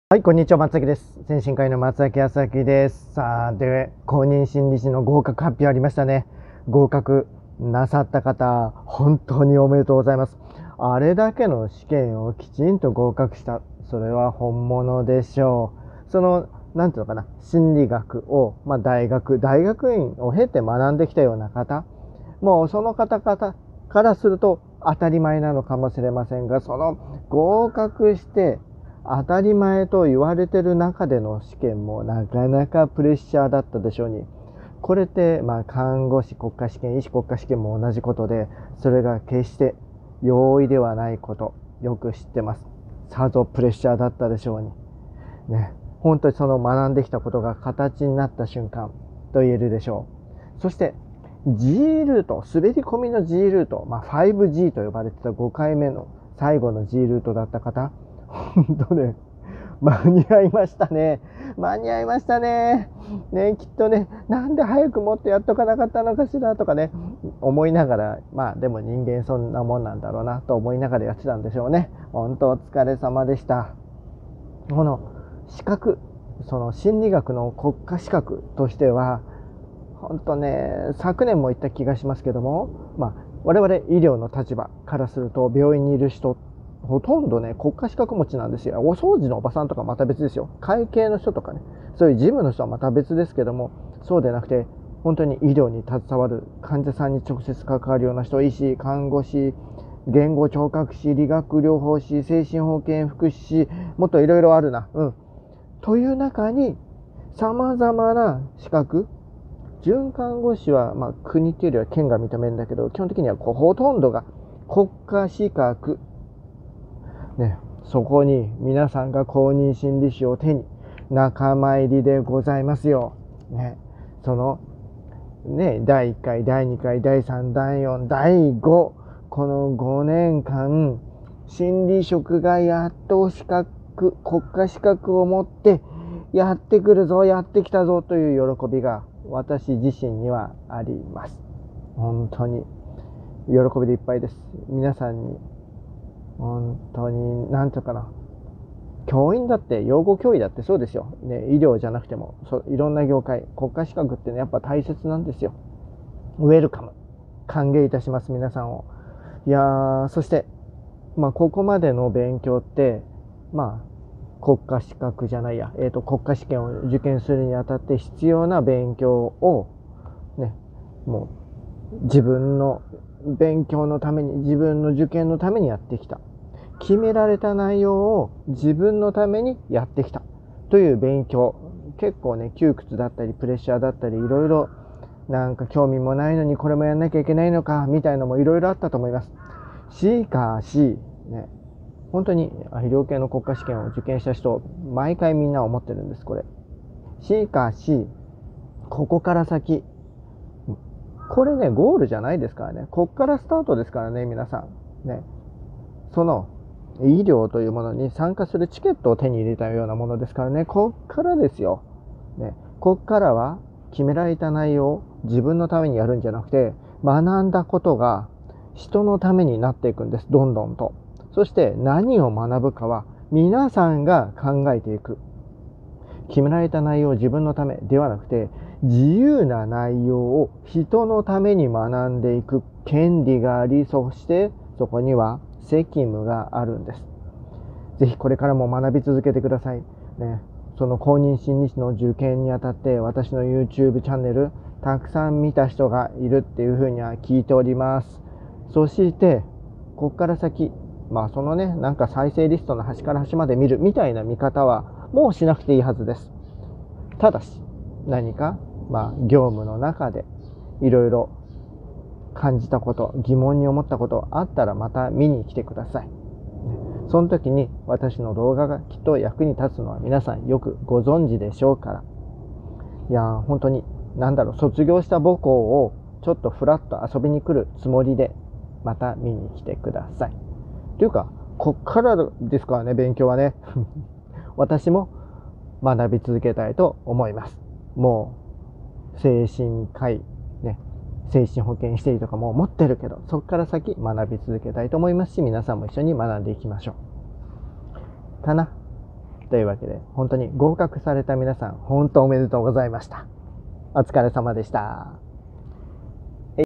公認心理師 合格者に向けて５分だけ喋らせて！
公認心理師に合格した皆さんを祝いたいだけの動画です。